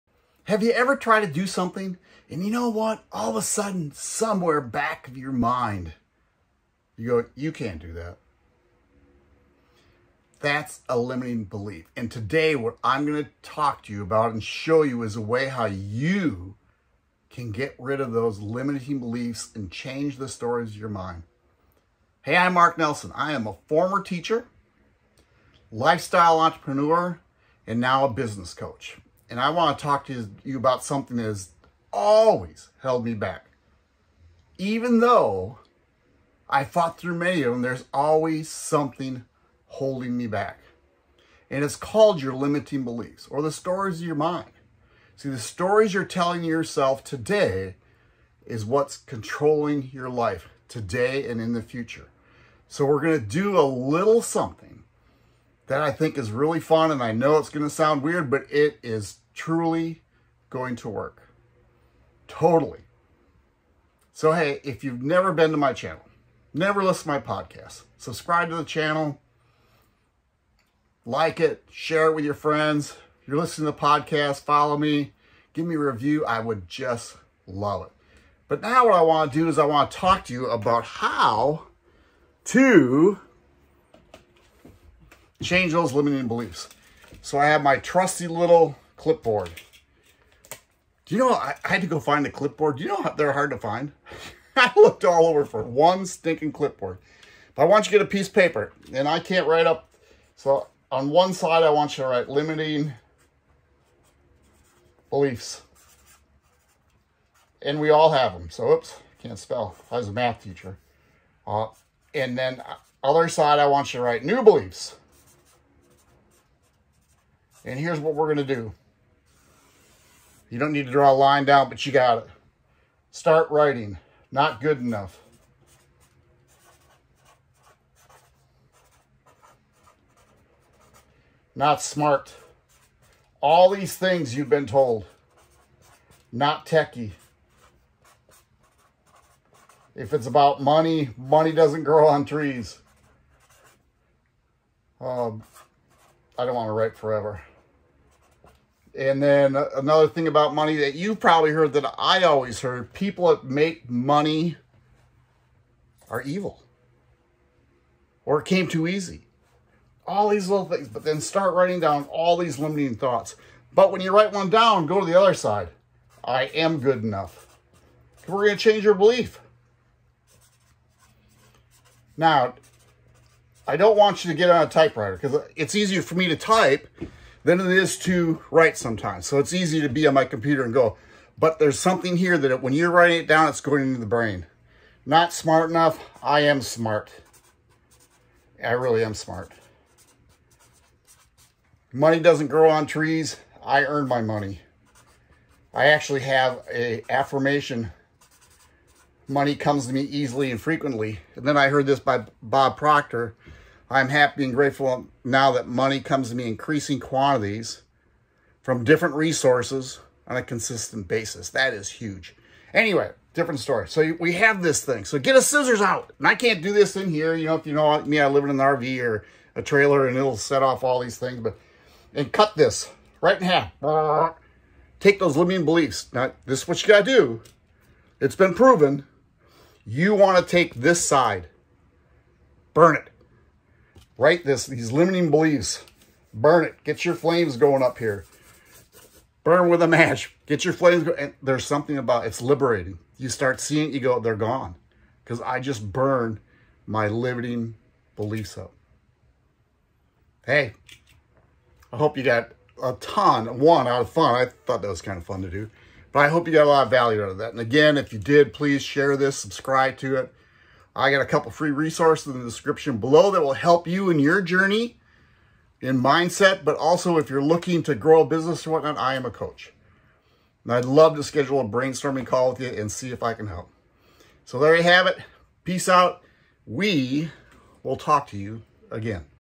How To Get Rid Of Limiting Beliefs
In today's video I go through an activity that will help you get rid of limiting beliefs.

Free Training On How To Create An Online Course